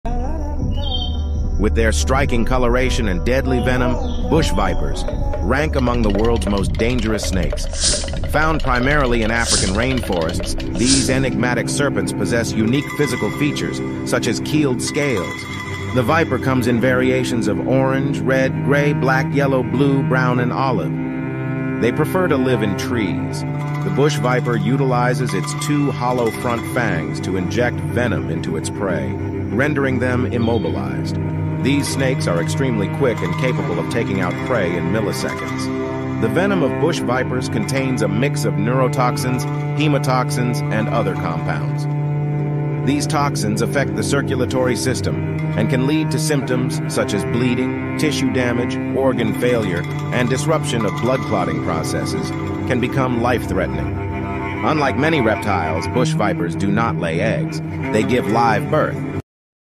Beauty and Deadly Venom: Bush Vipers
Explore the captivating world of bush vipers, where stunning beauty meets deadly venom. Witness their vibrant colors, unique physical features, and learn about the potent venom that makes them among the most dangerous snakes on Earth. Join us for an eye-opening journey into the mysterious realm of bush vipers!